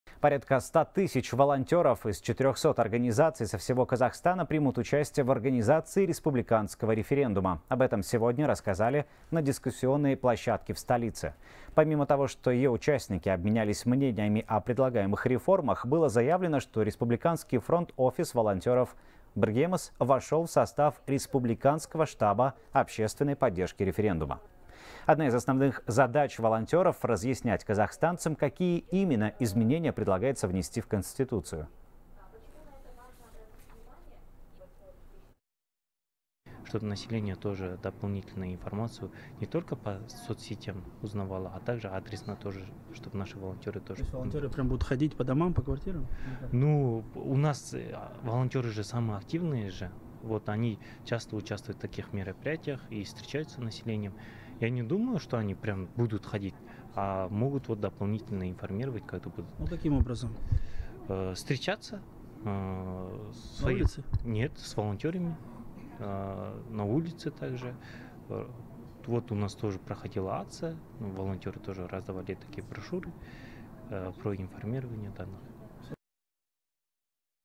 100 тысяч волонтёров будут помогать на референдуме
Порядка 100 тысяч волонтёров из 400 организаций со всего Казахстана примут участие в организации республиканского референдума. Об этом рассказали на дискуссионной площадке в столице, передает «Хабар 24».